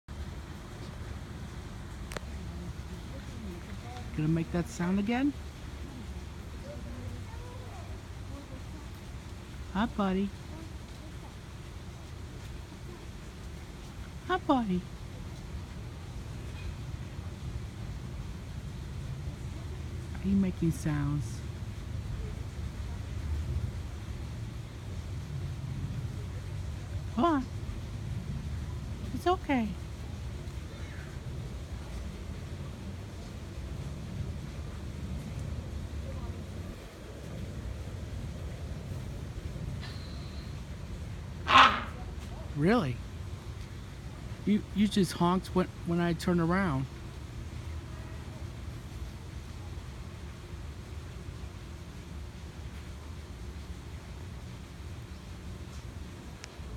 This Hornbill Won’t Honk if You Record Him
I was in the aviary in Reid Park Zoo when I heard this Hornbill honking. I recorded him and he only honked when I had the phone/camera pointed away from him.